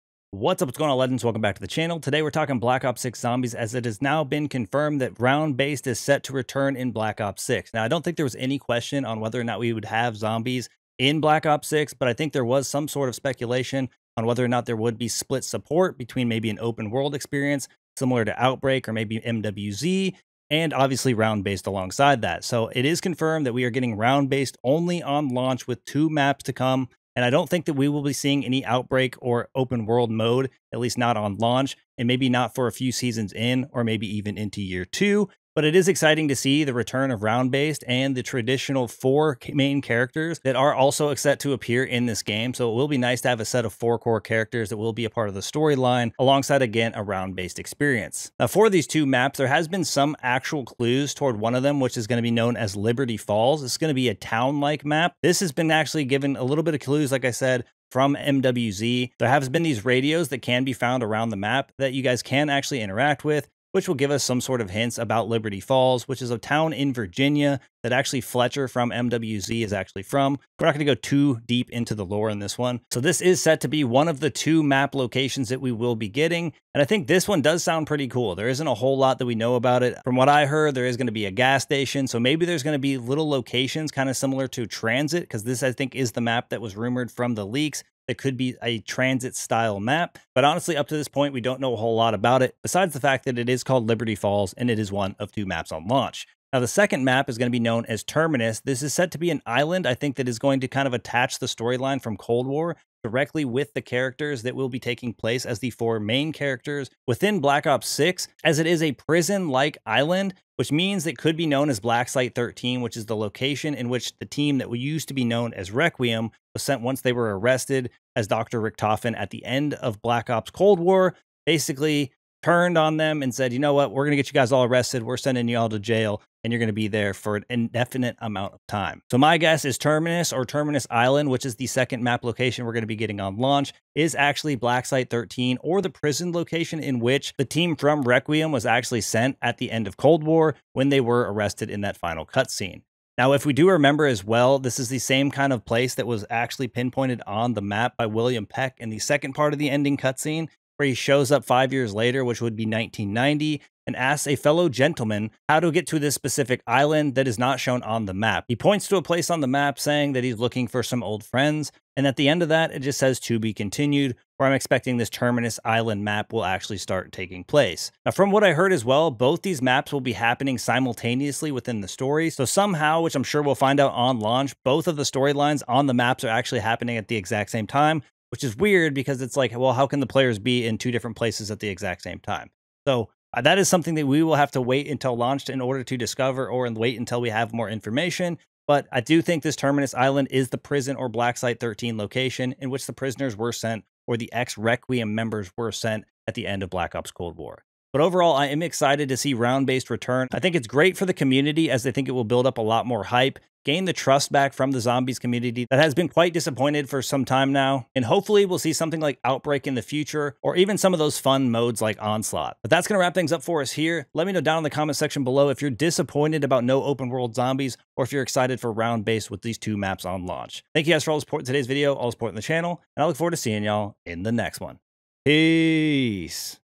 Round Based Only Confirmed For Black Ops 6 Zombies With 2 Maps Liberty Falls & Terminus On Launch
Round Based Only Confirmed For Black Ops 6 Zombies With 2 Maps Liberty Falls & Terminus On Launch. In today's video we talk about the return of round based zombies finally confirmed with 2 maps available at launch. Both liberty falls and terminus will be round based only with no signs of an open world zombies experience like outbreak. The story will continue directly after cold war with the final cutscene showing peck searching for his old friends who were likely prisoners on the new map terminus. With our new main crew I know I'm looking forward to the Easter Eggs these maps will have available on day 1 Thank you for all the support and I look forward to seeing you in the next one!

Looking to fine tune your aim?? I would be lost without my thumbsticks and precision rings

Chapters